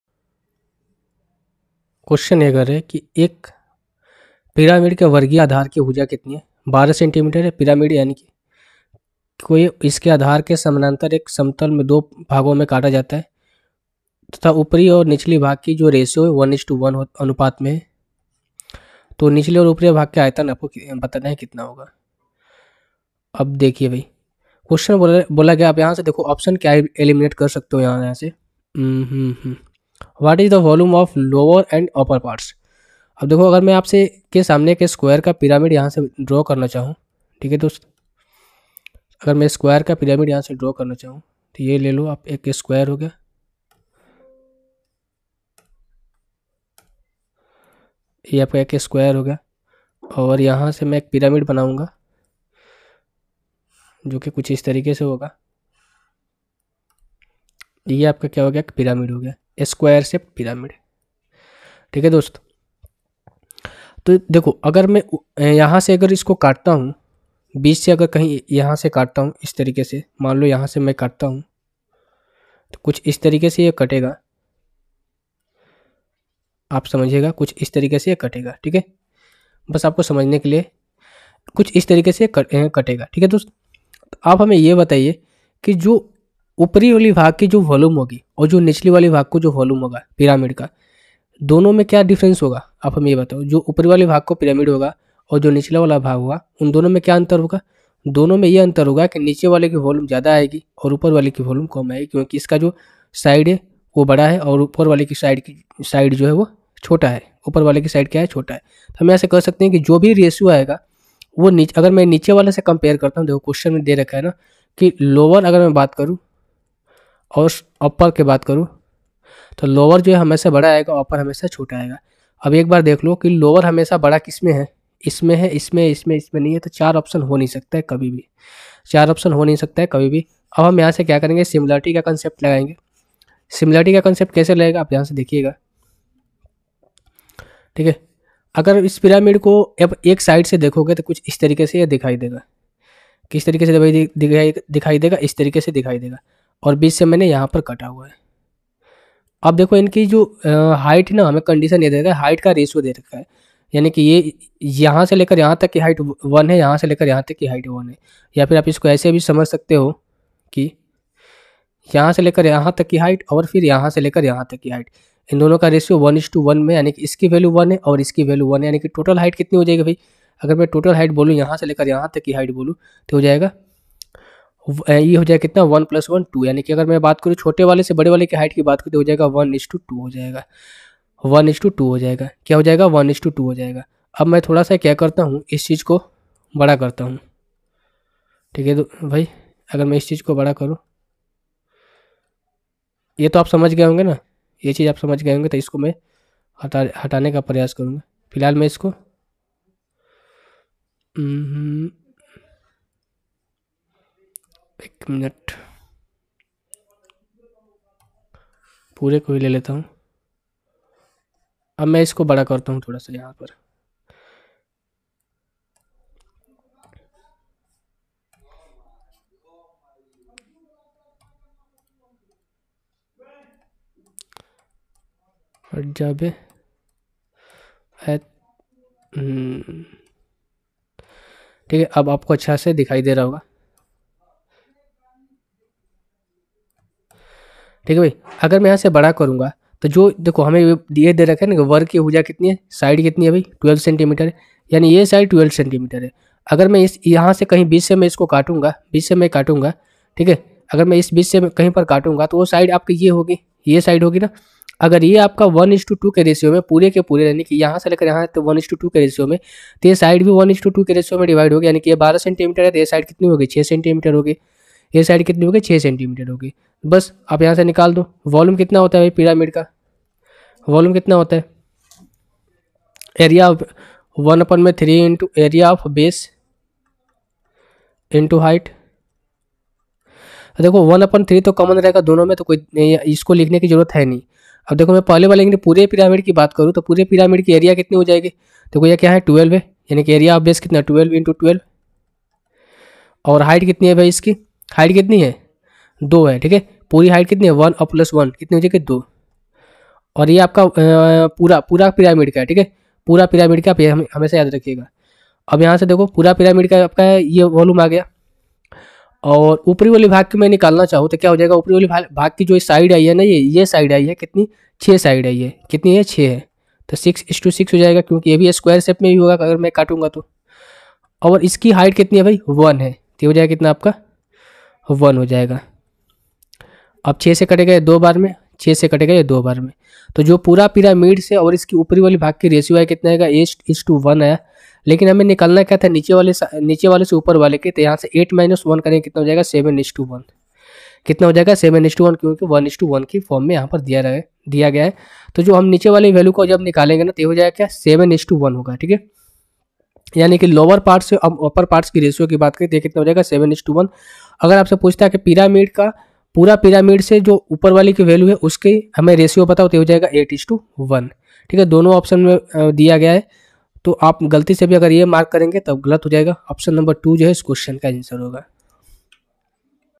Side of the square base of a pyramid is 12 cm. The pyramid is cut in two parts by a plane parallel..
Side of the square base of a pyramid is 12 cm. The pyramid is cut in two parts by a plane parallel to its base such that heights of upper and lower parts are in the ratio of 1 : 1. What is the ratio of volume of lower and upper parts respectively?
एक पिरामिड के वर्गीय आधार की भुजा 12 से.मी. है। पिरामिड को इसके आधार के समानांतर एक समतल से इस प्रकार दो भागों में काटा गया है कि ऊपरी तथा नीचले भाग की ऊँचाई 1:1 के अनुपात में है। क्रमशः नीचले तथा ऊपरी भागो के आयतन का अनुपात क्या है?